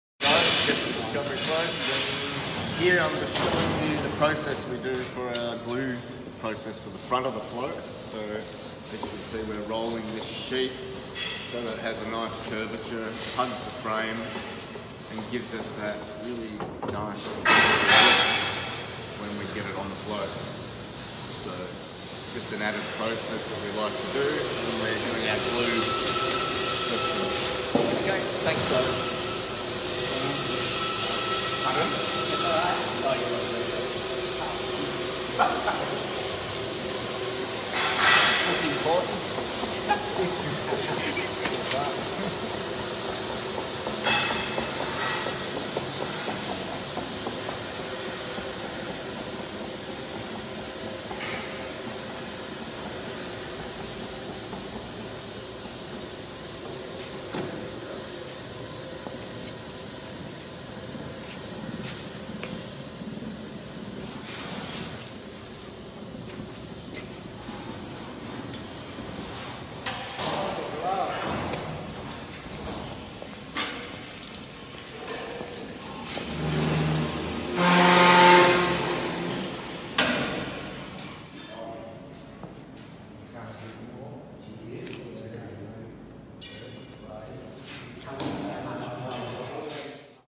Discovery Horse Floats Demonstrating Our Sheet Metal Process
This video live from Discovery Horse Floats HQ demonstrates how the team roll & press our Zincanneal sheets. This process creates the precise curvature for the front of our floats, ensuring a slimline fit that hugs tightly to the steel frame. Premium quality is paramount with every aspect of our all Australian build 🖤